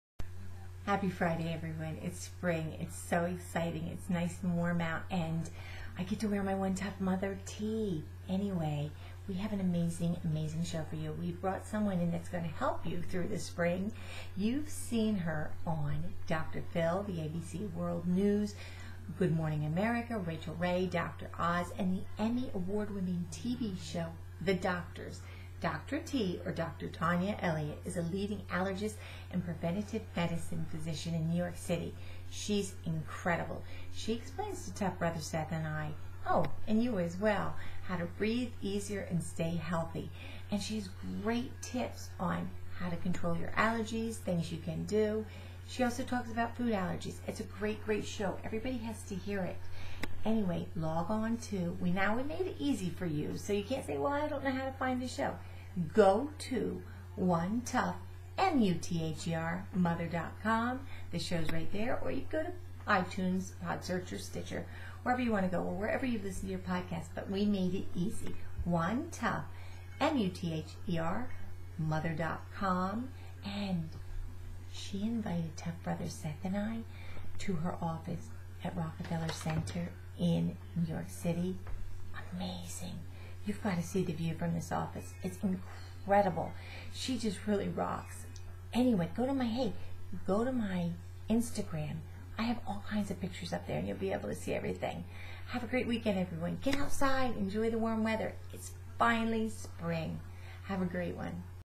OTM Show Guest Dr Tania Elliott
Dr T talks allergies, immune health, life, love and health hacks